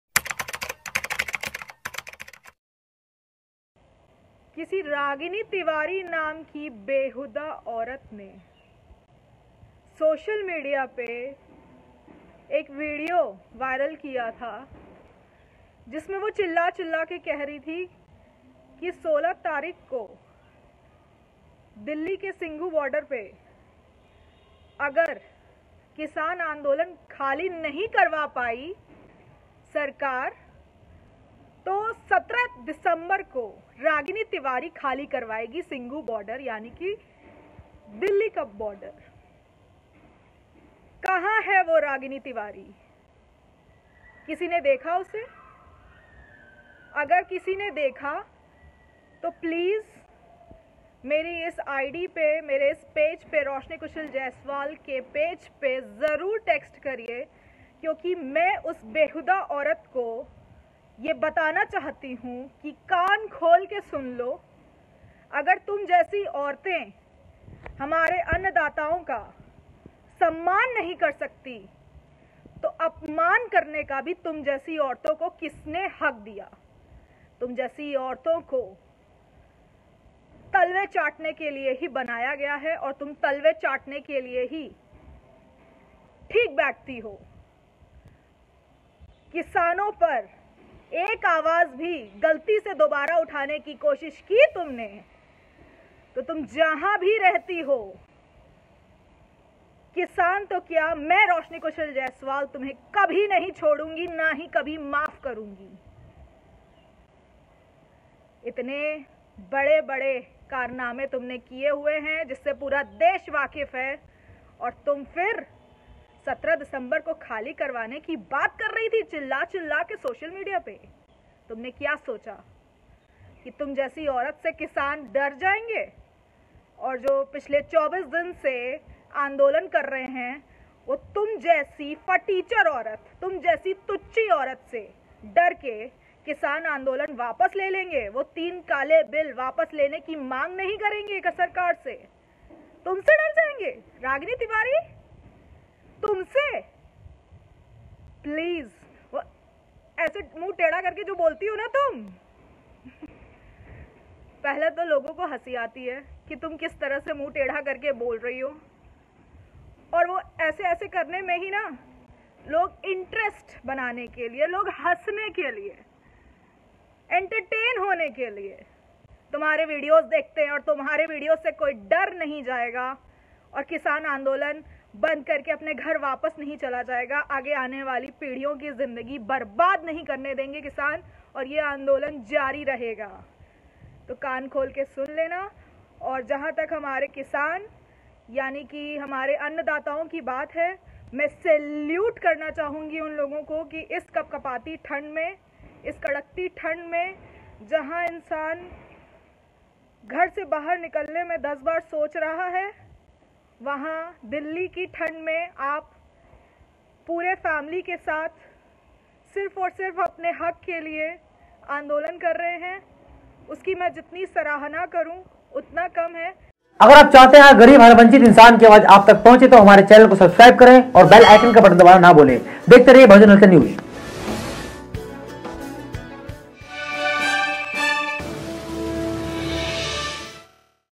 एक अभिनेत्री ने कैसे रागिनी तिवारी को किया बेनकाब! ROSHNI JAISWAL || RAGNI TIWARI || FARMERS PROTEST
Producer-
Editor- Karuna